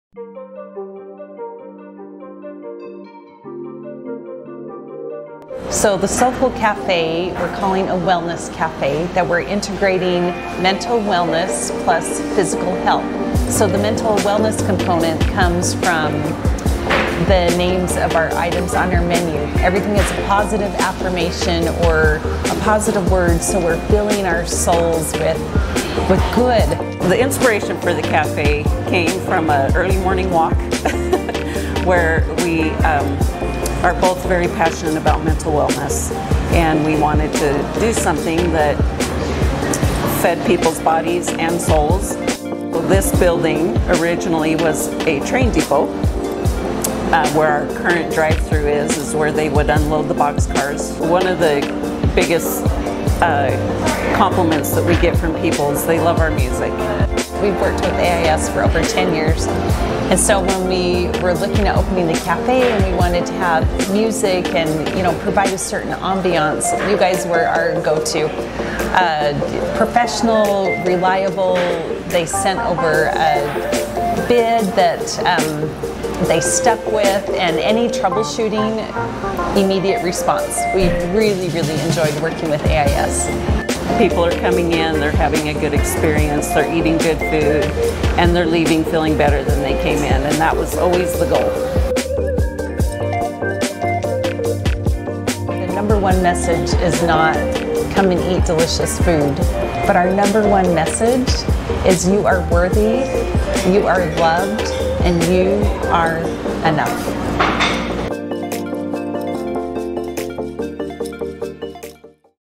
The SoulFull Experience ft. AIS
🎶🔒 Enhancing Safety and Serenity: Audio, Security, and Surveillance at a Wellness Cafe!

Welcome to a wellness cafe, where health and harmony meet technology and security! Join us as we unveil the innovative audio, security, and surveillance camera systems that ensure a safe and serene environment for our valued customers, both indoors and amidst the tranquil landscape outside.

In this enlightening video tour, you'll discover how we've integrated state-of-the-art technology to enhance the wellness experience for our guests, providing peace of mind and tranquility in every corner of the cafe.

🍃 Key Features of our Audio, Security, and Surveillance Systems:

Immersive Audio Experience: Immerse yourself in soothing sounds and uplifting music with our carefully curated audio system, designed to create a harmonious atmosphere that promotes relaxation and well-being.

Comprehensive Security Solutions: Rest assured knowing that your safety is our top priority with our comprehensive security solutions, including strategically placed surveillance cameras and advanced alarm systems that provide round-the-clock protection for the cafe and its customers.

Surveillance Camera Technology: Gain peace of mind with our state-of-the-art surveillance camera technology, offering high-definition video monitoring both indoors and outdoors to deter theft, vandalism, and ensure the safety of the guests and staff.

Audio Integration in the Landscape: Extend the wellness experience outdoors with seamlessly integrated audio technology in our landscape, allowing guests to enjoy the sounds of nature and soothing music while dining outside.

Remote Monitoring and Control: Stay connected and in control with remote monitoring and control capabilities, giving staff the ability to monitor audio levels, security cameras, and alarm systems from any location for added convenience and peace of mind.

🌟 Experience the perfect blend of wellness and technology at this beautiful cafe!